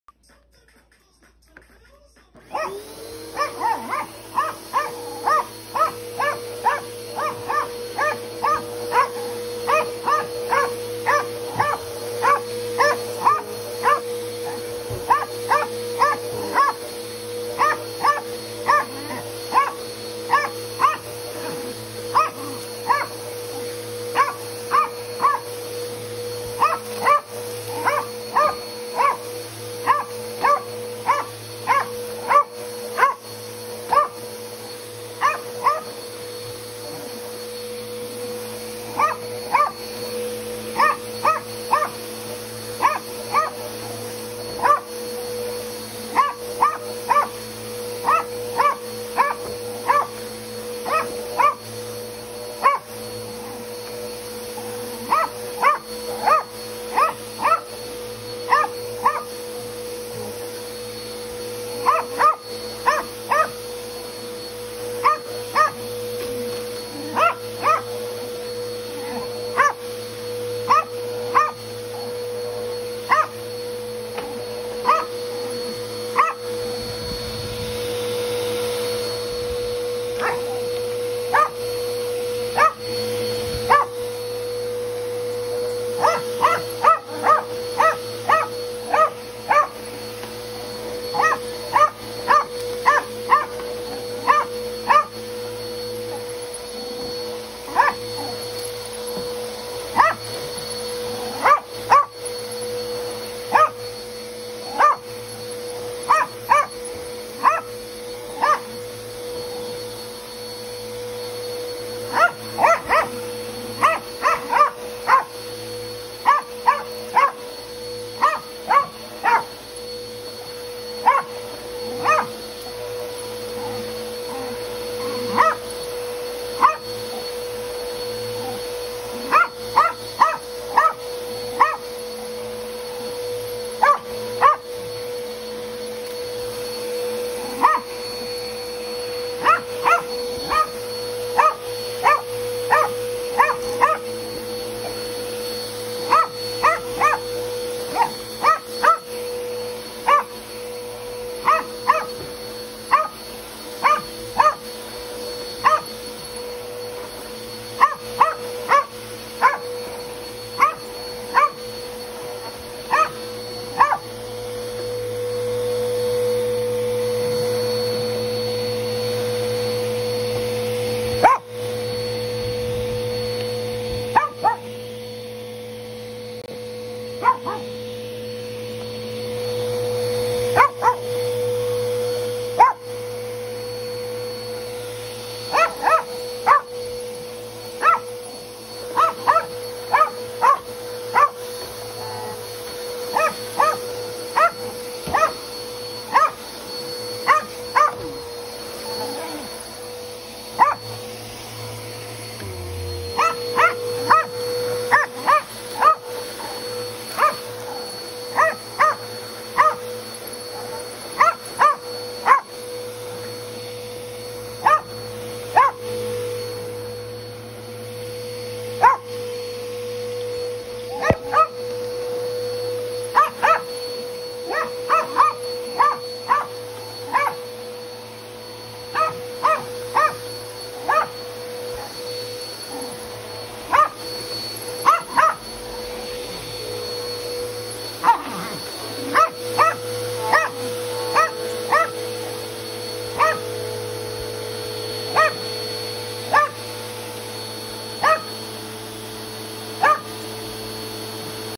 Pooey and the vacuum cleaner..
I try to wait as long as possible to vacuum and this is why..